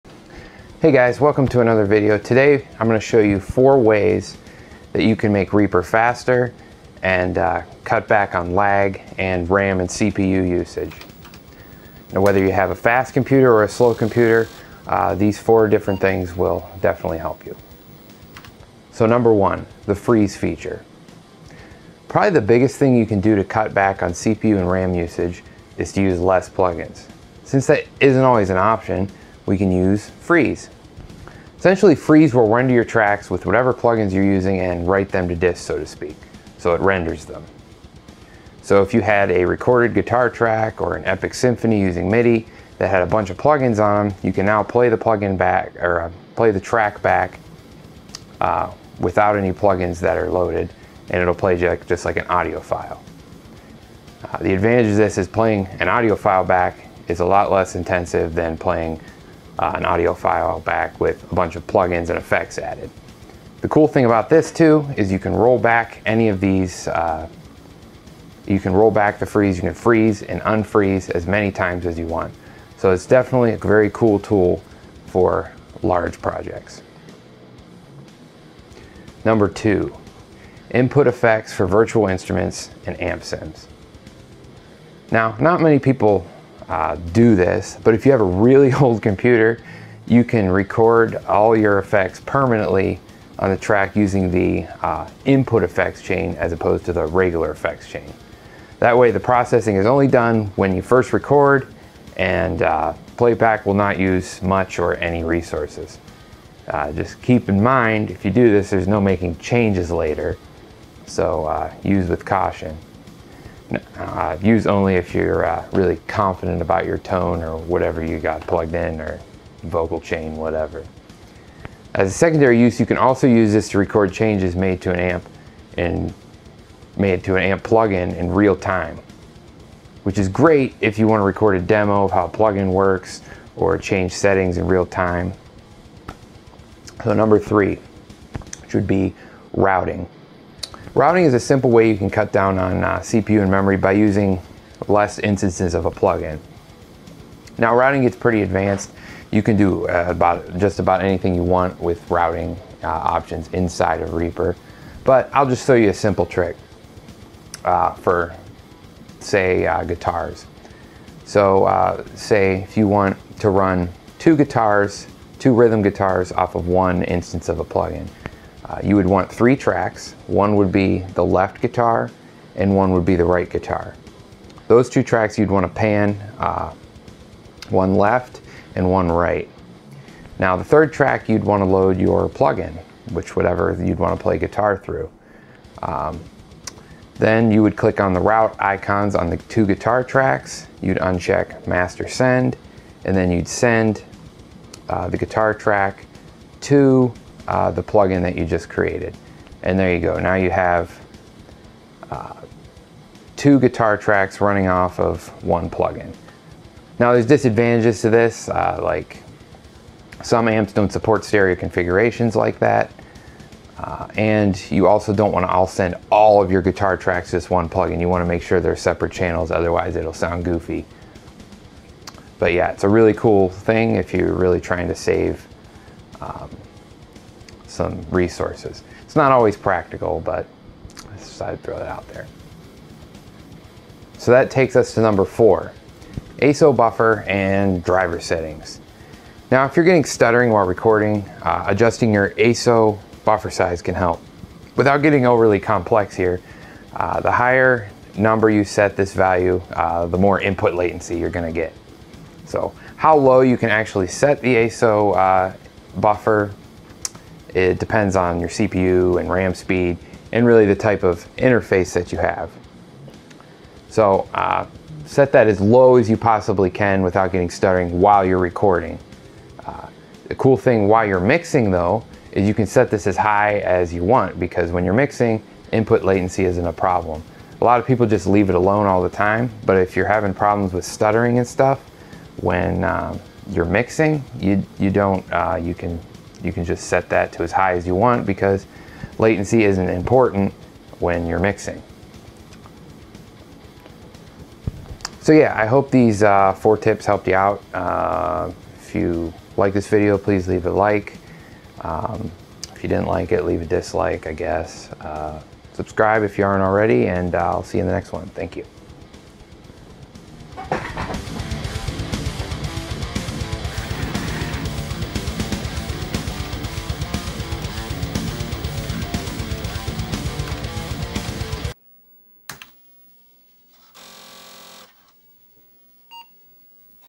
REAPER: 4 Ways to stop plugins from eating your CPU and memory
So you have 4,000 plugins loaded and Reaper runs like hot garbage? I got Ya covered. Hopefully. Let me know what your favorite tricks are for making your DAW run better!